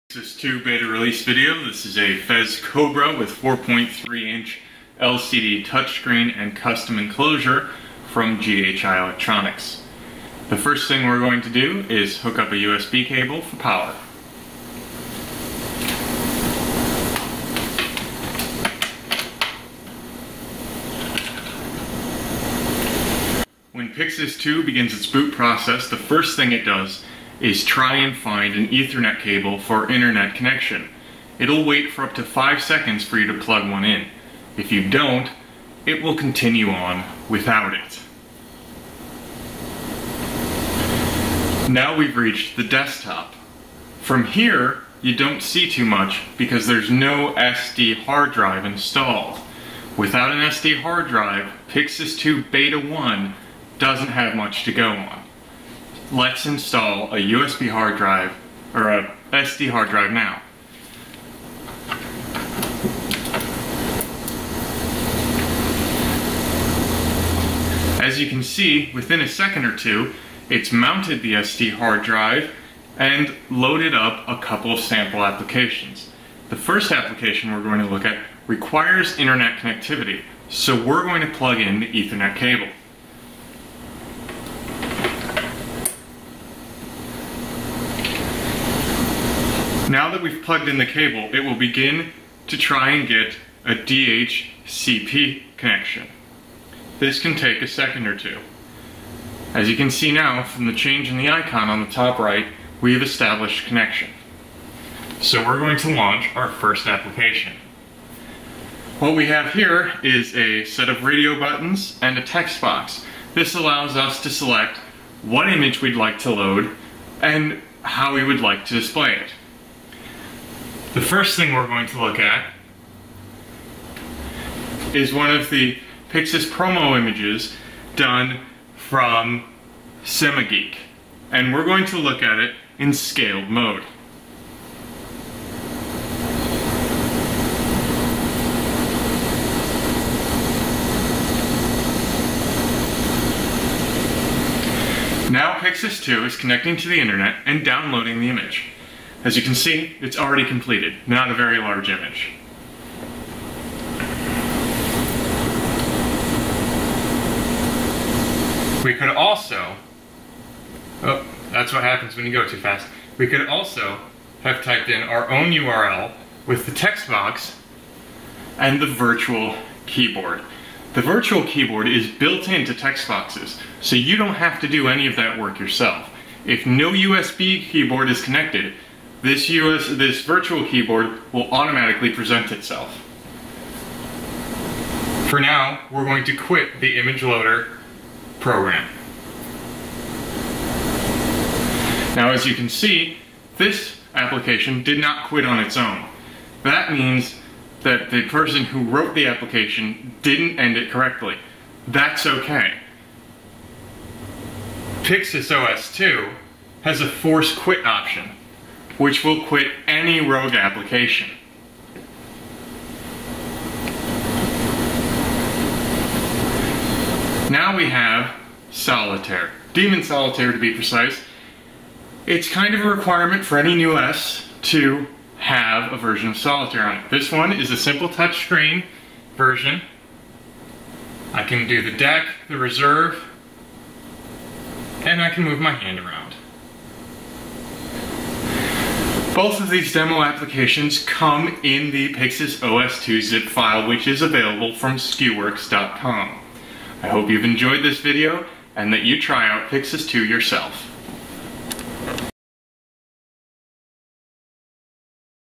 Pyxis 2.0 Beta Released!
If you've been waiting for a completely free version of Pyxis OS that runs Microsoft .NET MicroFramework (NETMF) code, your wait is over! The Pyxis 2.0 beta has been released and is ready for you to load on your FEZ Cobra with LCD.

Enjoy full access to touch-screen interface, SD cards, internet, threads, and much more! Right out of the box you'll be able to see images being downloaded over the web to a picturebox or play a nice game of Demon Solitaire. You can also begin developing your own applications! And since we use NETMF your source code is protected from other people viewing.